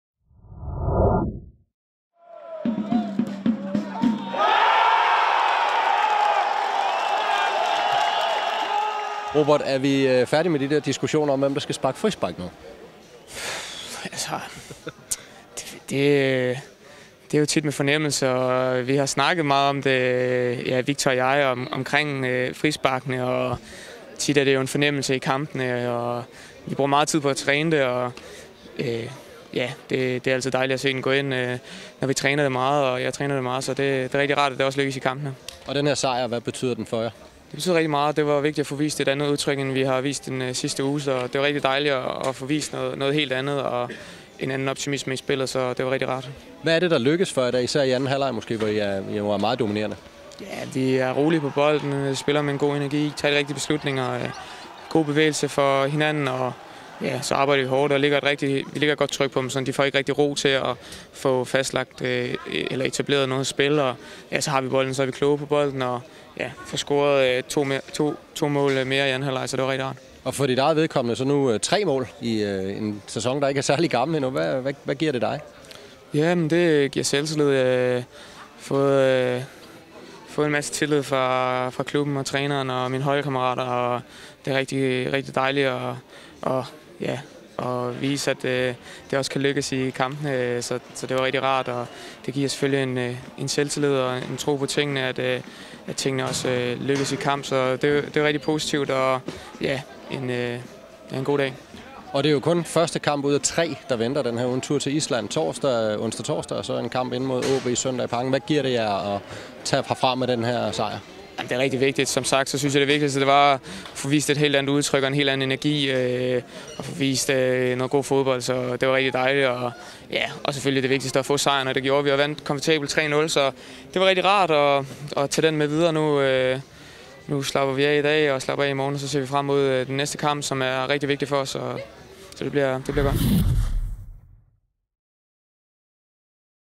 Tomålsskytten Skov: Vi viser et helt andet udtryk
Robert Skov scorede to mål i 3-0-sejren over Hobro - blandt andet en flot frisparkskasse. Vi fangede vores beskedne nummer 29 efter sejren.

profilnavn: fc_kobenhavn